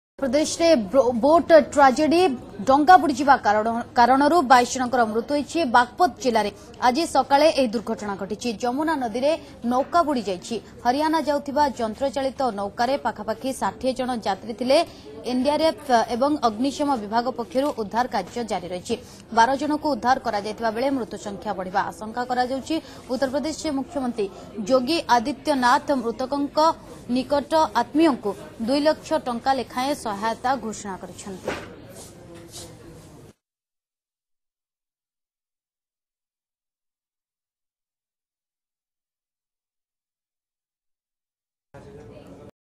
ଉତ୍ତରପ୍ରଦେଶ ବାଗପତ ଜିଲ୍ଲାରେ ଯମୁନା ନଦୀରେ ନୌକା ବୁଡି ୨୨ ରୁ ଅଧିକ ନିଖୋଜ.
ଉତ୍ତରପ୍ରଦେଶ ବାଗପତ ଜିଲ୍ଲାରେ ବୁଡିଗଲା ଡଙ୍ଗା... ଯମୁନା ନଦୀରେ ନୌକା ବୁଡି ୨୨ ରୁ ଅଧିକ ନିଖୋଜ...ଏନଡିଆରଏଫ ପକ୍ଷରୁ ଉଦ୍ଧାର କାର୍ଯ୍ୟ ଜାରି ... ମୃତକଙ୍କ ପରିବାରକୁ ସହୟତା ଘୋଷଣା କଲେ ୟୁପି ମୁଖ୍ୟମନ୍ତ୍ରୀ